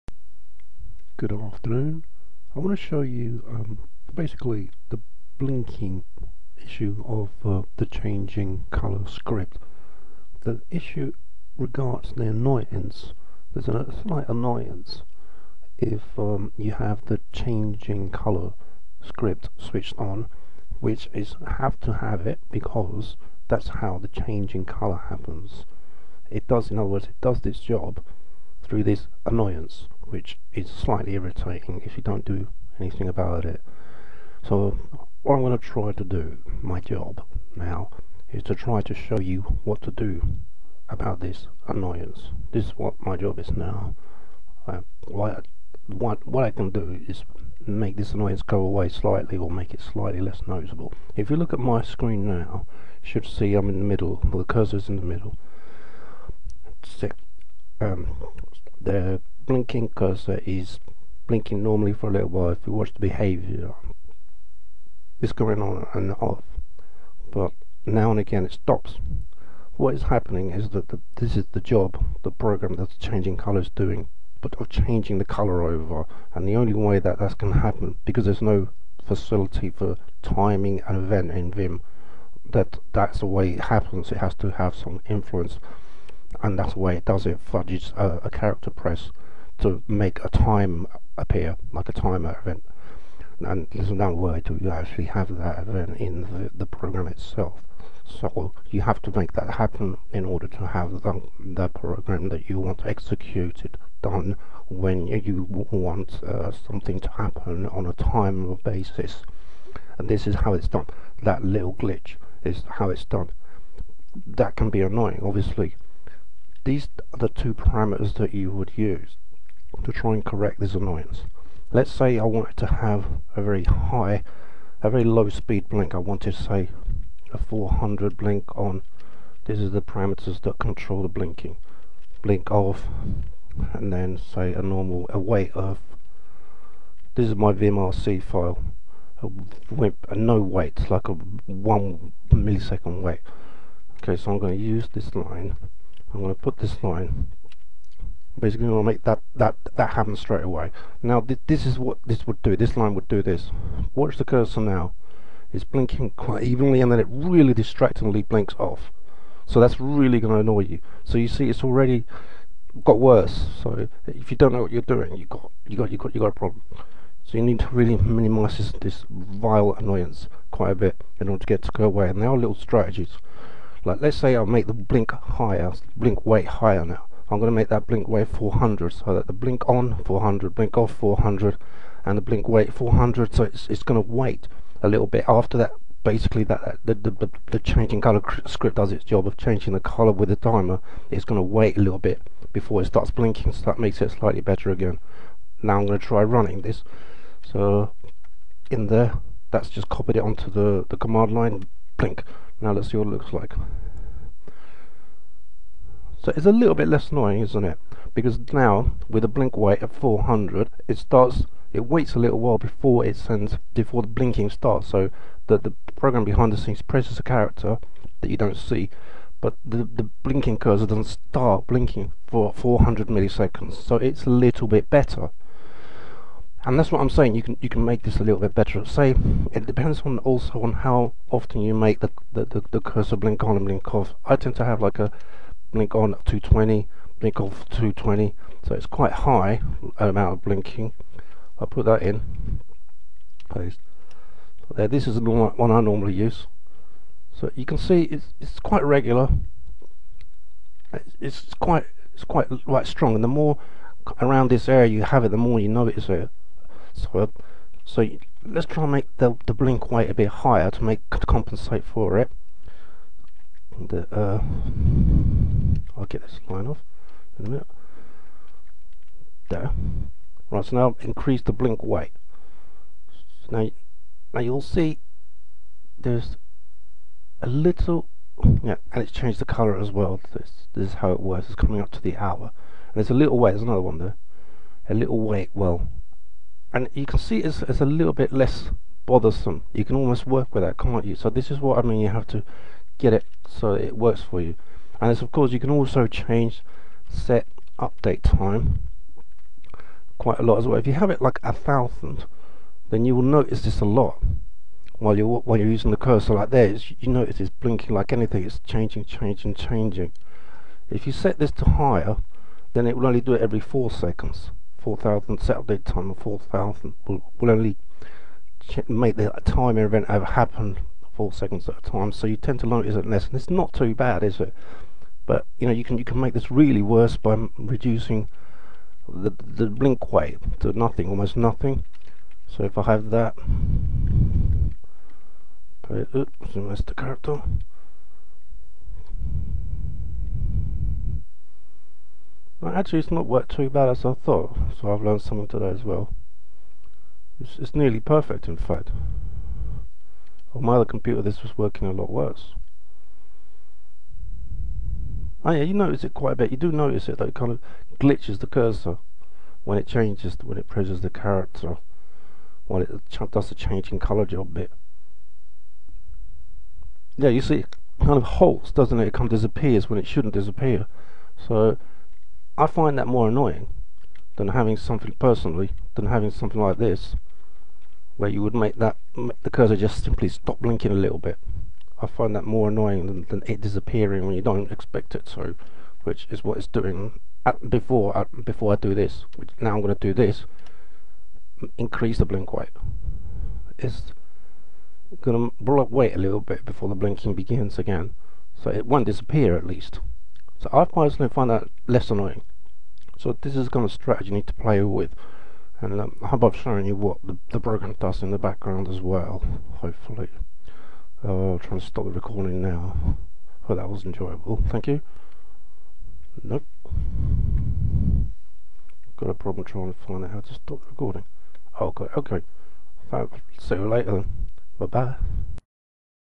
cursor-blink-annoy.mp4
Explanation of cursor blinking 'glitch' caused by changing color vim script and strategies to overcome it, or prevent it becoming annoying. Also shows changing color script in action.